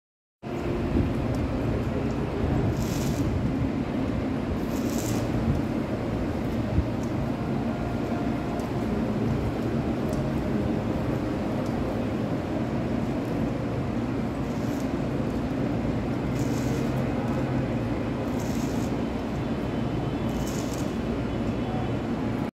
新自強EMU3000 雨刷噴水清潔擋風玻璃 2023.12.31 [鐵道風情84] /EMU3000 Wiping Windshield in the Sun /台鉄特急 EMU3000 ワイパー動画
錄影車輛：ED3111 (第11編組/ 3110 之第1車)
Vehicle number: ED3111 (Car 1 of the 11th set / 3110)